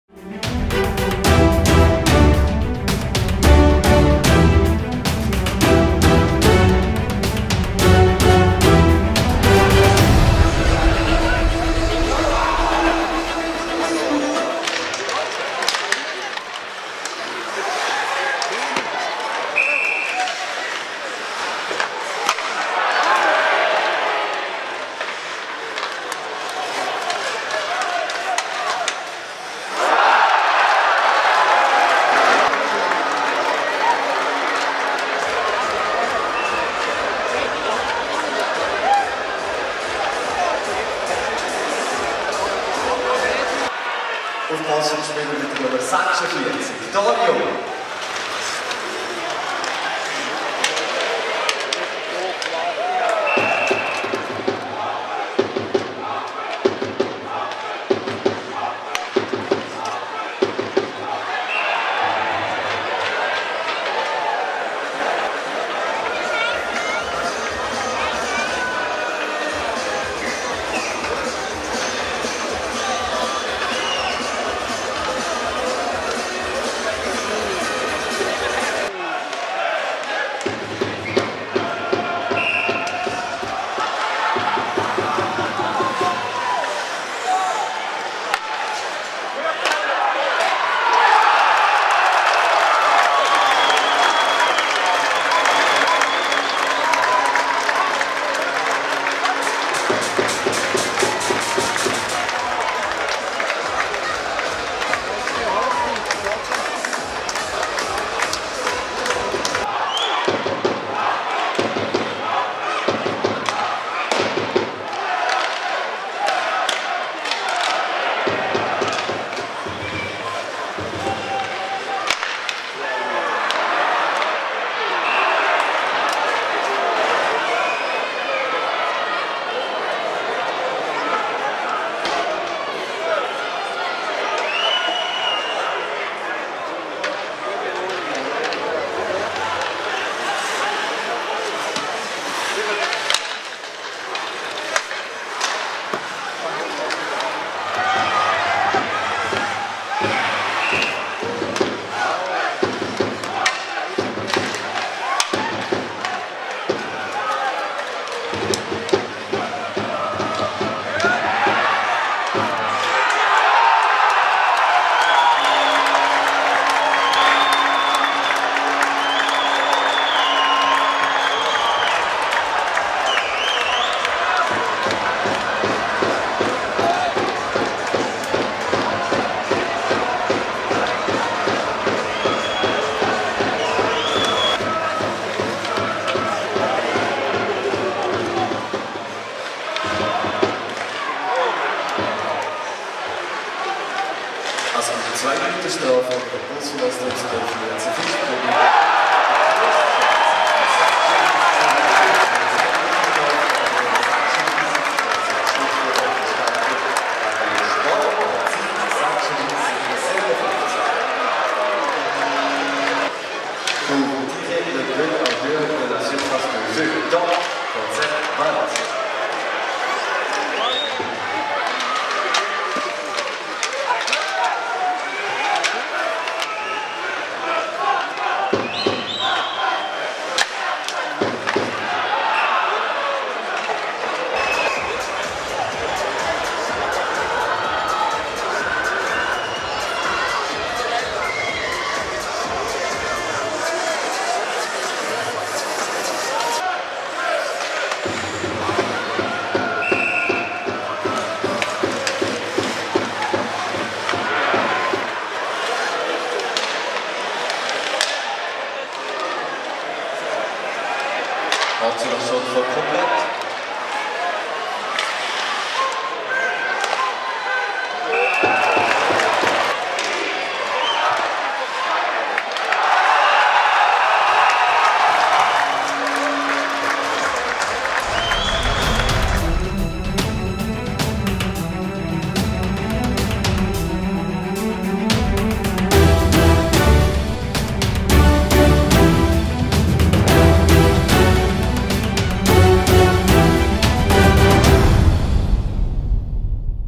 13.01.2017 EHC Viège - HC La Chaux-de-Fonds (4-3)
Résumé du 37ème match du championnat de LNB 16-17.
EHC Viège  - HC La Chaux-de-Fonds (4-3) du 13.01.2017
Vidéo & Montage : Fragnoli Fausto cameraman officiel du HCC